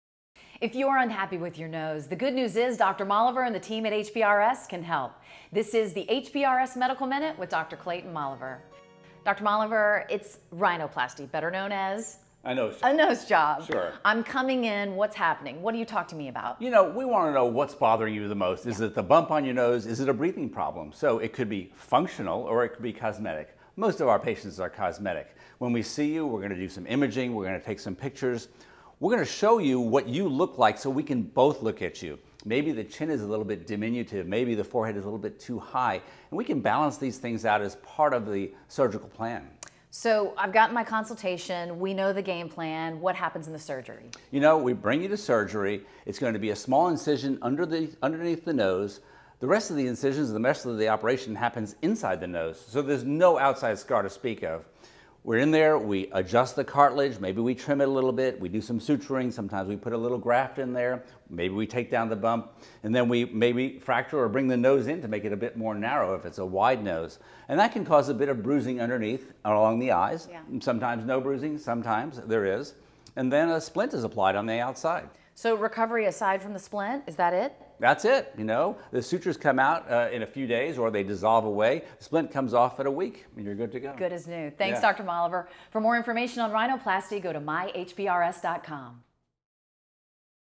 HPRS Medical Minute: Rhinoplasty
Here Dr. Moliver describes rhinoplasty consultations and surgery at one of his three locations, Webster Clear Lake, Museum District Houston, and Pearland.